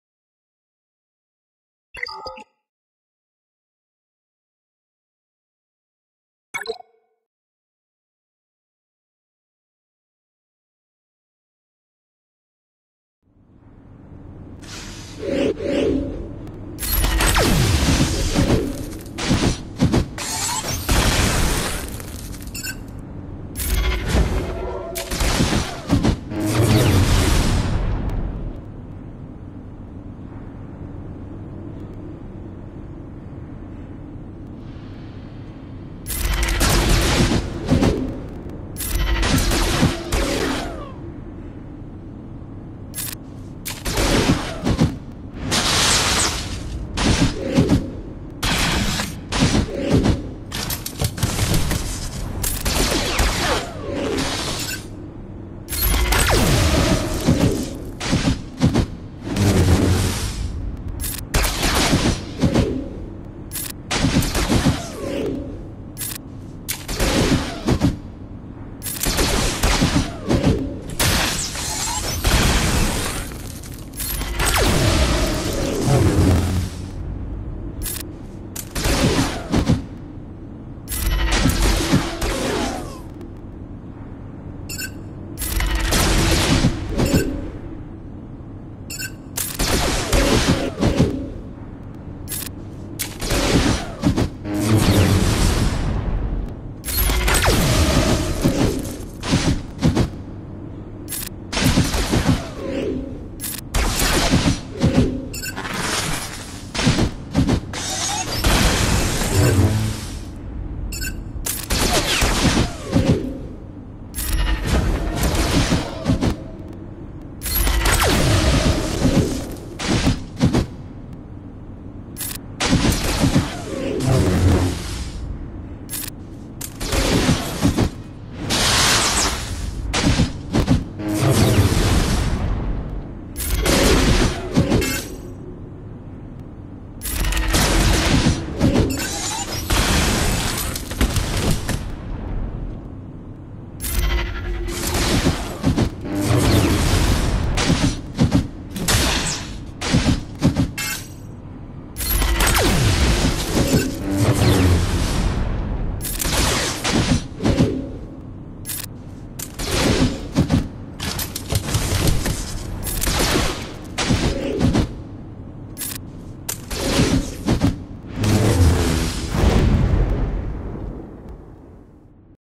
[3v3] Mon Mothma/Cassian/K-2SO fails vs. GG/B2/Magna (Tank revive, 110% defence): 2 banners
This MM squad is usually an extremely reliable counter against all GG squads that don't have Wat (which nobody uses anymore).

The datacron here cause me to fail, and I think the 110% defence was more of the culprit than the revive. It just took so long to get them down.

I probably made a mistake by taking B2 into red and the switching focus to Magna. I wanted to be able to kill him quickly after the Magna revive, but this wasn't a good idea as it let GG take too many turns in quick succession. I should have just focused on one character at a time.